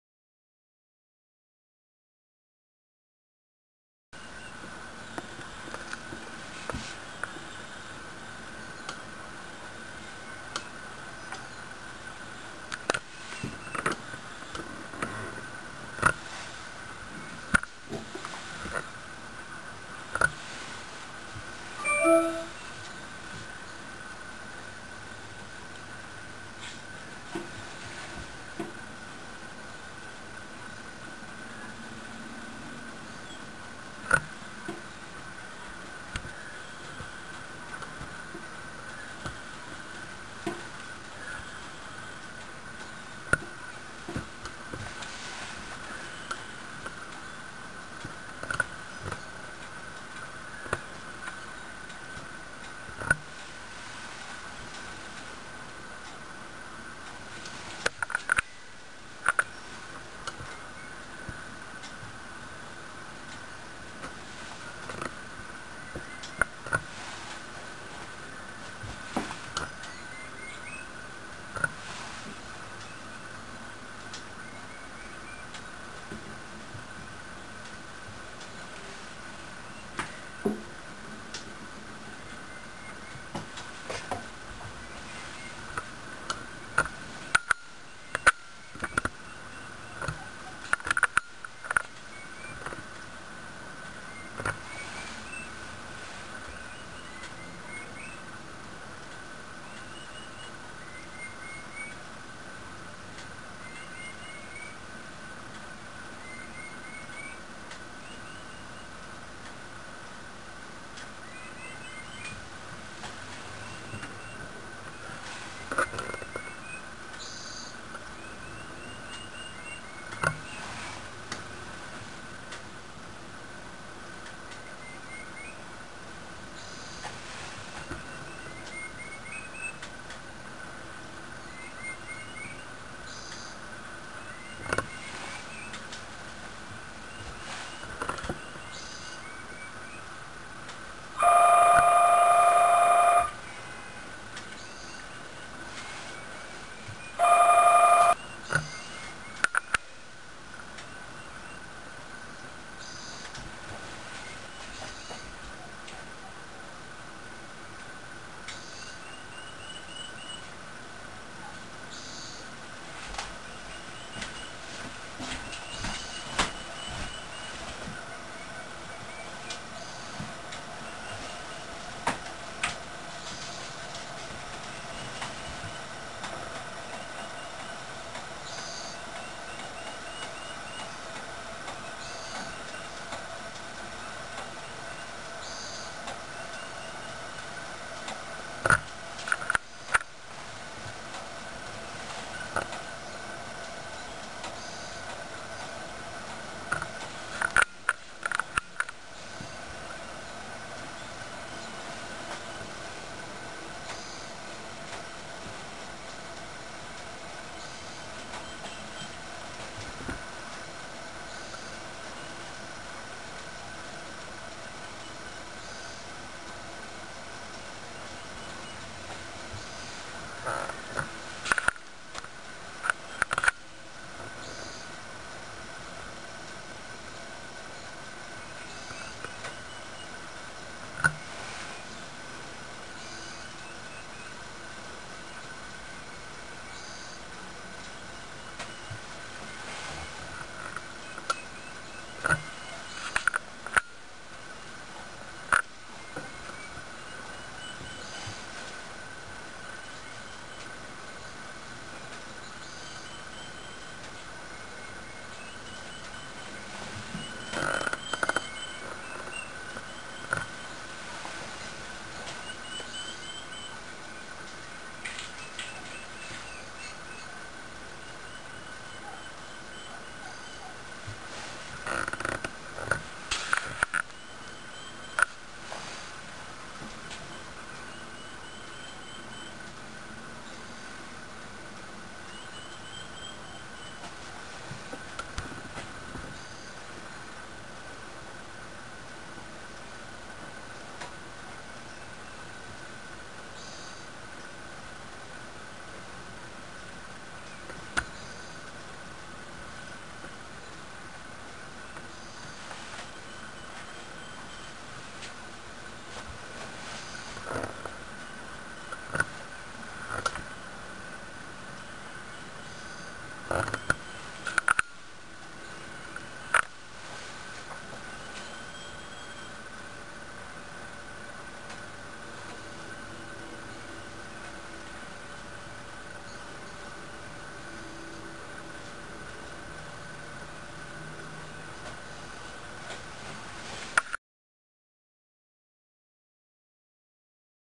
Wild Turkeys Visit
A large family group of wild turkeys arrives for a late afternoon visit. There are at least 6 adult females and 8 or more babies, some bigger than others.